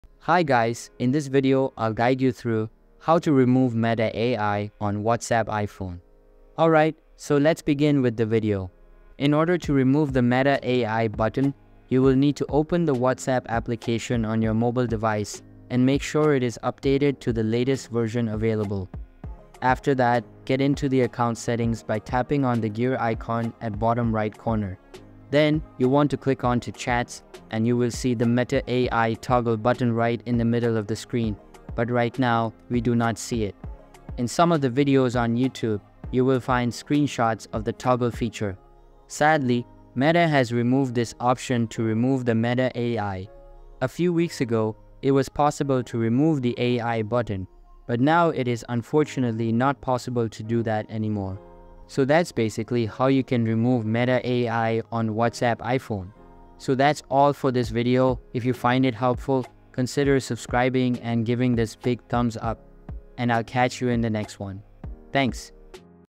How To Remove Meta AI on WhatsApp iPhone [easy]
In this video, I'll show you how to remove Meta AI from WhatsApp on your iPhone. Begin by opening the WhatsApp app and navigating to your chat list. Find the chat with Meta AI and swipe left on it to reveal the "More" options. Tap on "More," then select "Delete Chat" to remove the conversation. To ensure Meta AI is fully removed, go to WhatsApp settings, tap on "Account," and then "Privacy" to check for any remaining linked contacts or settings related to Meta AI. If you encounter any issues, restarting your iPhone or reinstalling WhatsApp can help reset the app.